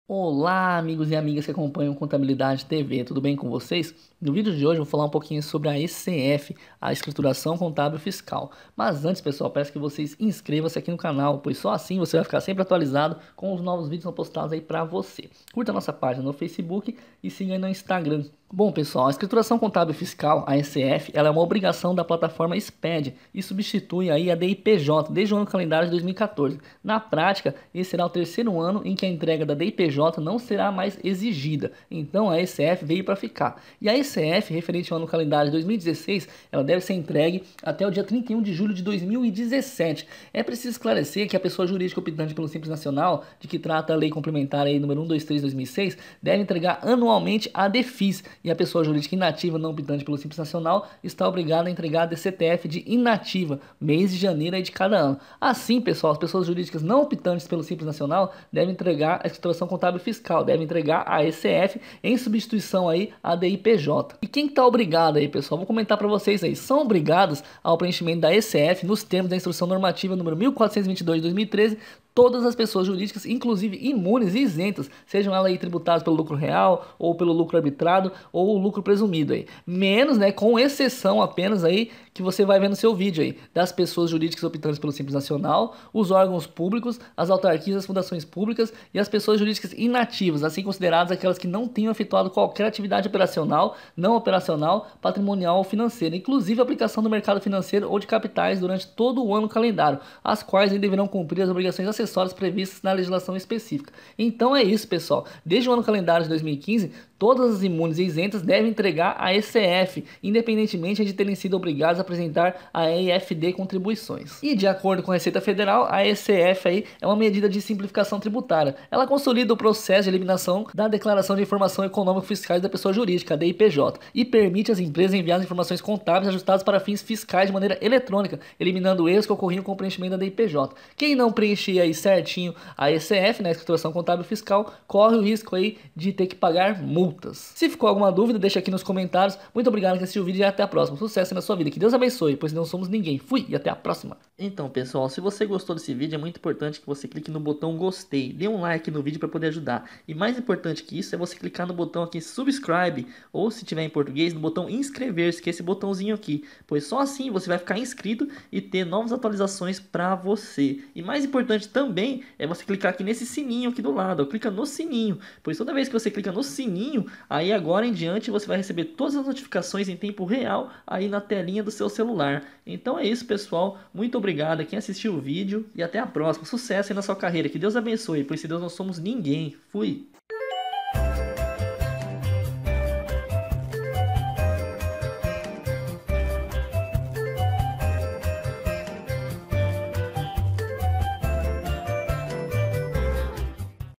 🔴 ECF – obrigatoriedade e multas
A Escrituração Contábil Fiscal (ECF), é uma obrigação da plataforma  SPED, e substitui a DIPJ desde o ano-calendário 2014. Saiba quem esta obrigado a fazer a declaração!